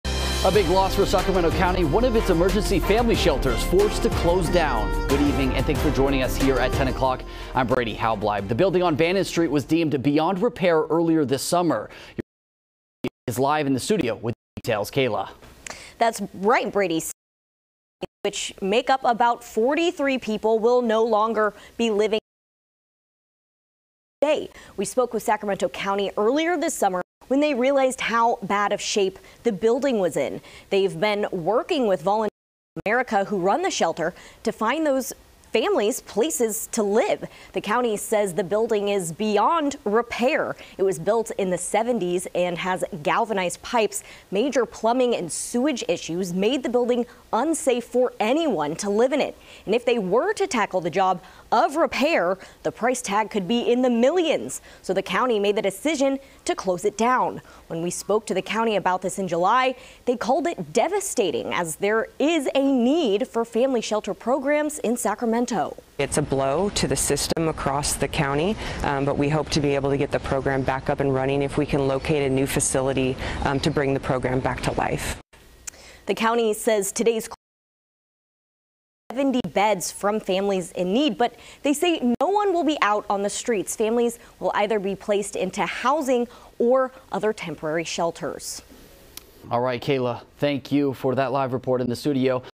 Sacramento emergency family shelters closes it doors
An emergency family shelter closed its doors on Saturday after the building was deemed beyond repair earlier this summer.